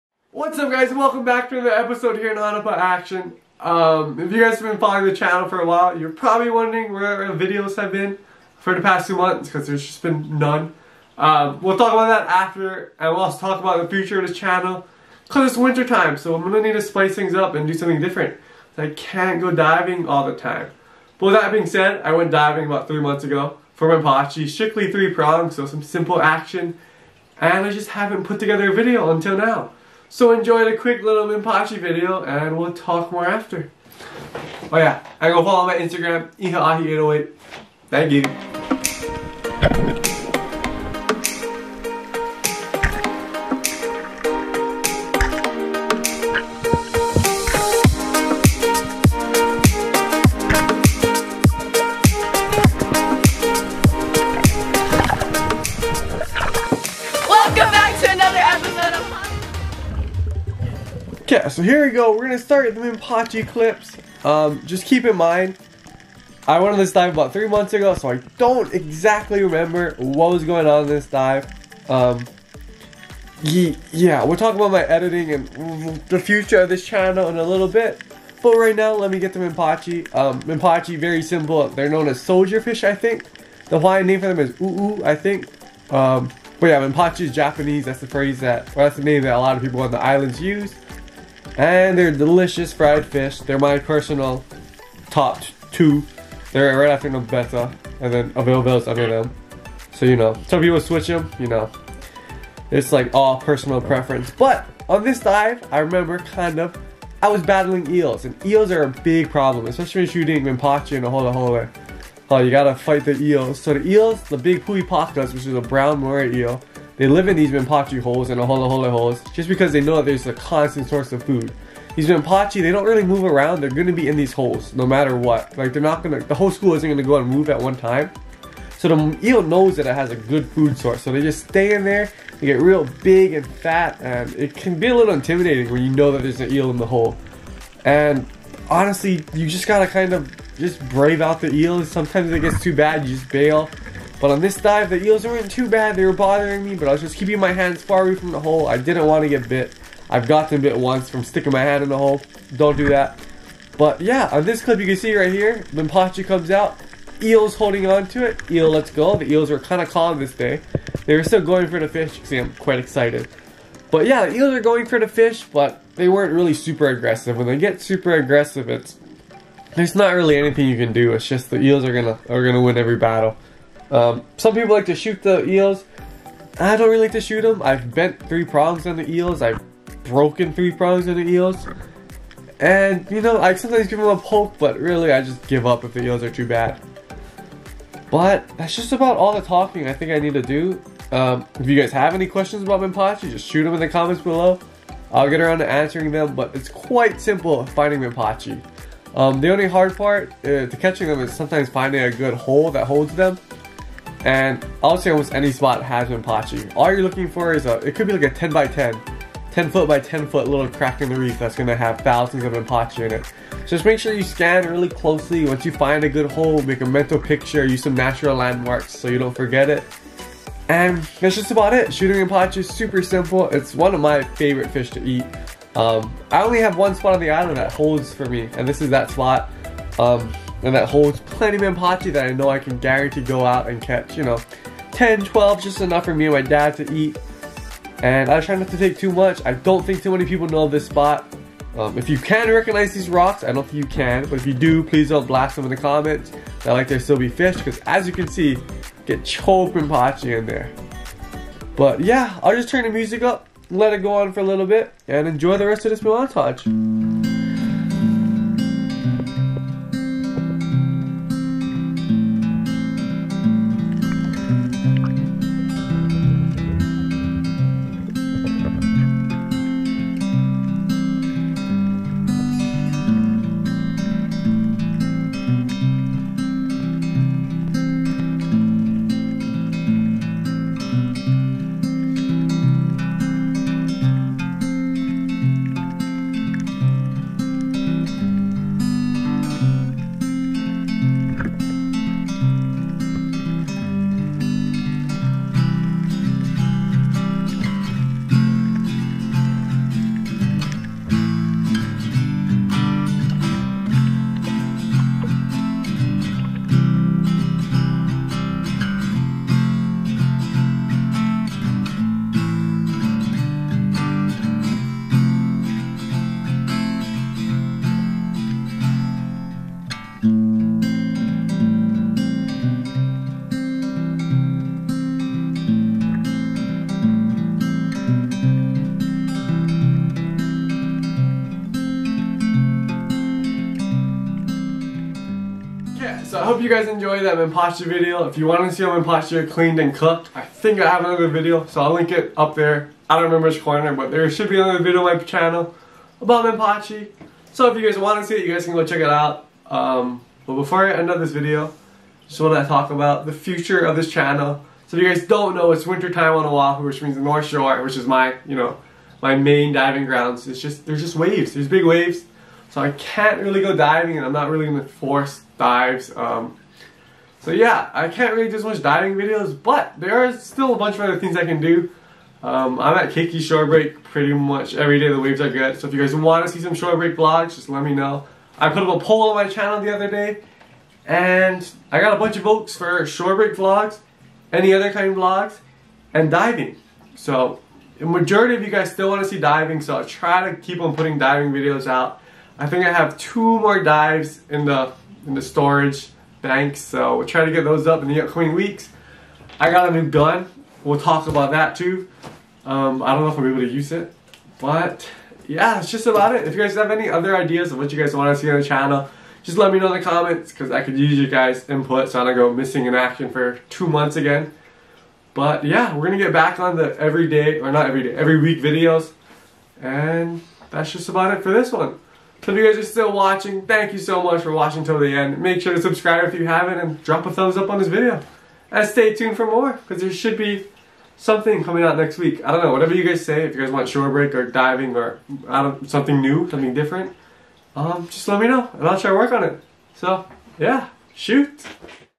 FIGHTING EELS FOR MENPACHI three prong diving hawaii
In this episode I go diving for menpachi with my trusty three prong and end up having to battle eels to secure my dinner.